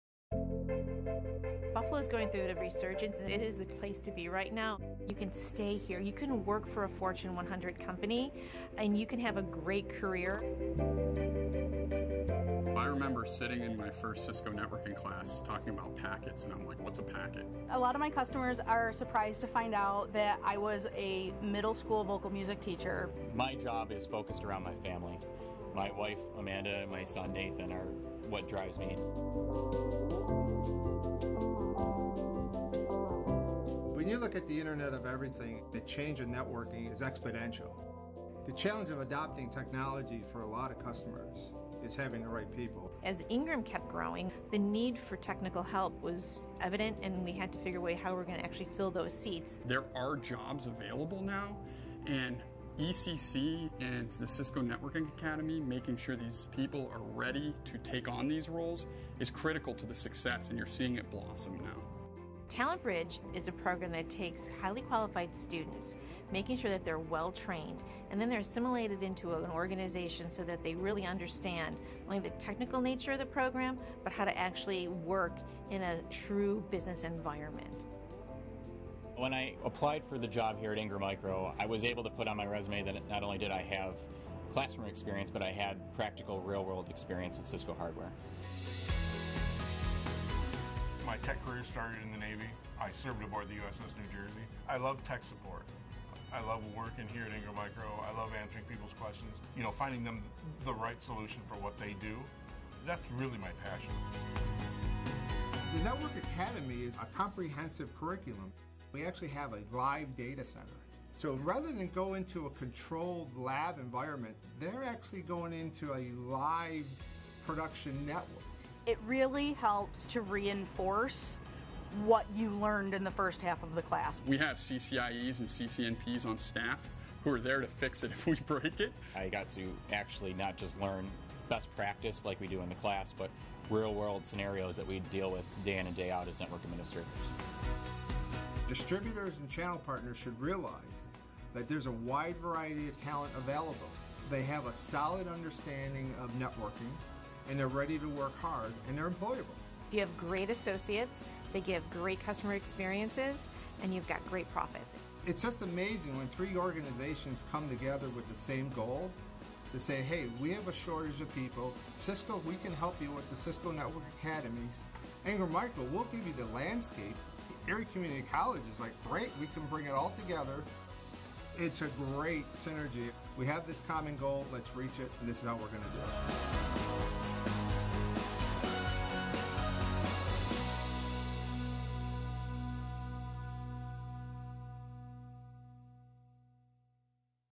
Erie Community College Partners with Cisco and Ingram Micro
The Cisco Networking Academy is a comprehensive e-learning program that provides students with the information technology skills essential in a global economy.  The Networking Academy program delivers Web-based content, online assessment, student performance tracking, hands-on labs, instructor training and support.  The program also prepares individuals for industry-standard certifications like Cisco Certified Entry Networking Technician (CCENT), Cisco Certified Network Associates (CCNA), Cisco Certified Network Associate Security (CCNA Security), Cisco Certified Network Professional (CCNP) and CompTIA A+.